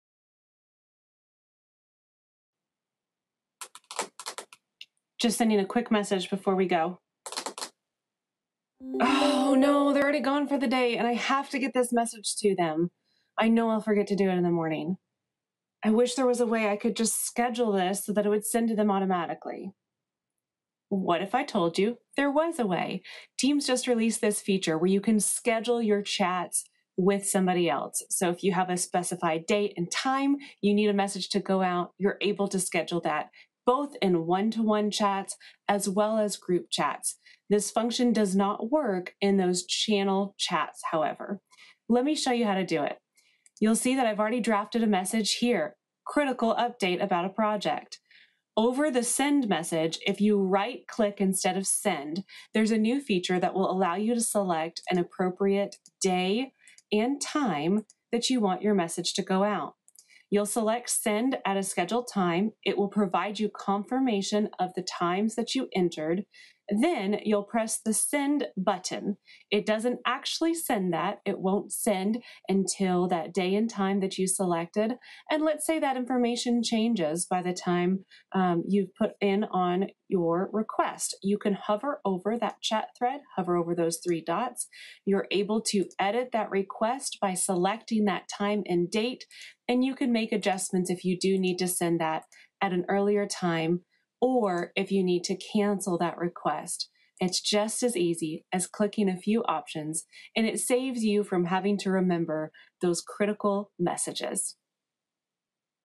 How to Easily Schedule a Teams Chat Messages
Looking to schedule a @Microsoft Teams Chat message for later? Don't worry, we have you covered! In this video, we'll show you how to schedule a message for later using the Teams interface.

In this video, we'll show you how to schedule a message for later using the Teams interface, so you can stay connected with your team even if you're off the clock!